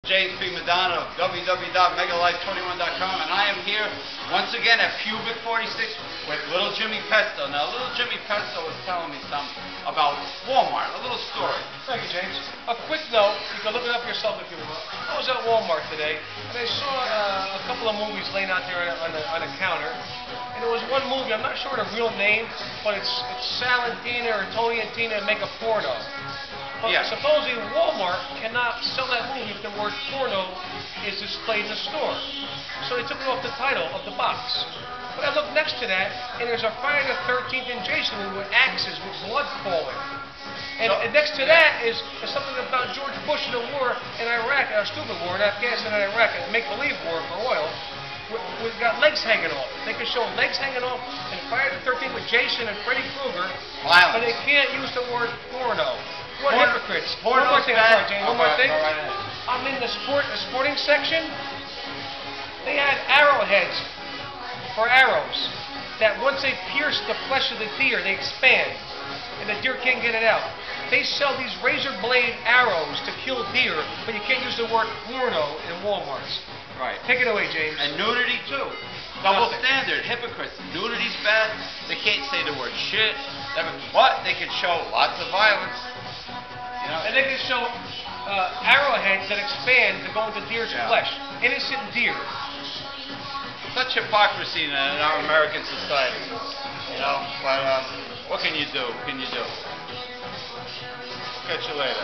Megalife21's James with "lil" Jimmy Pesto at Pub 46
"lil" Jimmy Pesto shares his words of wisdom concerning the double standard hypocrisies of modern American society by telling a story about an observation at Walmart.